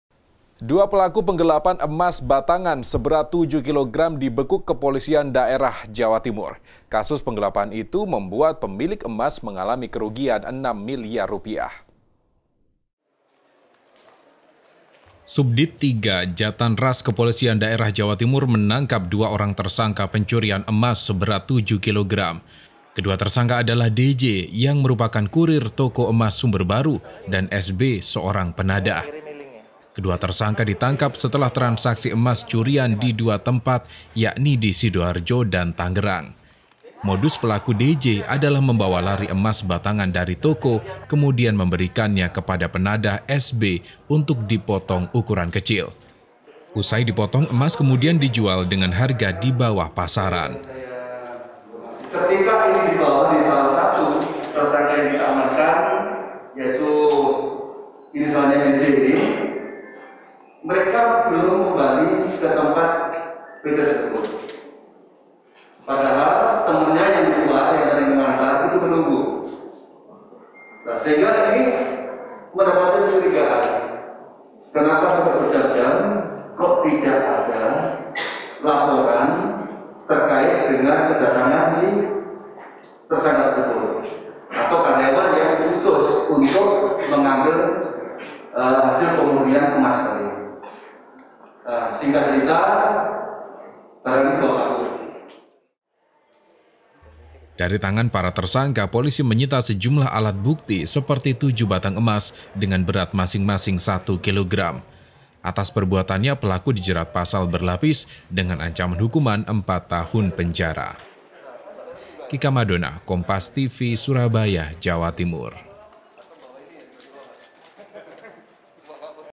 Pelaku Penggelapan Emas Batangan 7 Kilogram Dibekuk
SURABAYA, KOMPAS.TV - 2 pelaku penggelapan emas batangan seberat 7 kilogram dibekuk Kepolisian Daerah Jawa Timur. Kasus penggelapan itu membuat pemilik emas mengalami kerugian 6 miliar rupiah.

Subdit III Jatanras Kepolisian Daerah Jawa Timur menangkap dua orang tersangka pencurian emas batangan seberat 7 kilogram.

Baca Juga Eks Manajer Denny Sumargo Buka Suara soal Dugaan Penggelapan Uang di

Kedua tersangka adalah D-J, yang merupakan kurir toko emas sumber baru dan S-B yang merupakan seorang penadah. Kedua tersangka ditangkap setelah transaksi emas curian di 2 tempat berbeda, yakni di Sidoarjo dan Tangerang.

Wakapolda Jatim, Brigjen Slamet Hadi Supraptoyo menjelaskan modus pelaku, yakni pelaku D-J membawa lari emas batangan dari toko, kemudian memberikannya kepada penadah S-B untuk dipotong dengan ukuran kecil. Usai dipotong, emas kemudian dijual dengan harga di bawah pasaran.

Dari tangan  kedua tersangka, polisi menyita sejumlah alat bukti, seperti 7 batang emas dengan berat masing-masing satu kilogram.

Baca Juga Perempuan Jadi Dalang Penggelapan Mobil Sewaan, Polisi Amankan 40 Unit Mobil! di

Atas perbuatannya, pelaku dijerat pasal berlapis dengan ancaman hukuman 4 tahun penjara.